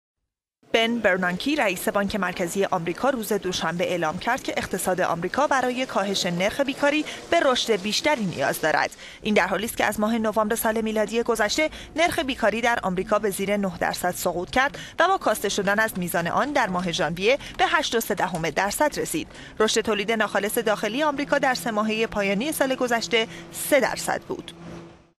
بن برنانکی خواستار توجه به تسریع رشد اقتصادی شد
بن برنانکی، رئیس بانک مرکزی آمریکا روز دوشنبه اعلام کرد که اقتصاد آمریکا برای کاهش نرخ بیکاری به رشد بیشتری نیاز دارد.

این در حالیست که از ماه نوامبر سال میلادی گذشته نرخ بیکاری در آمریکا به زیر نه درصد سقوط کرد و با کاسته شدن از میزان در ماه ژانویه به 8.3 درصد رسید.

رشد تولید ناخالص داخلی آمریکا در سه ماهه پایانی سال گذشته به سه درصد رسید که پیش بینی می شود در سال جاری به دو درصد کاهش یابد.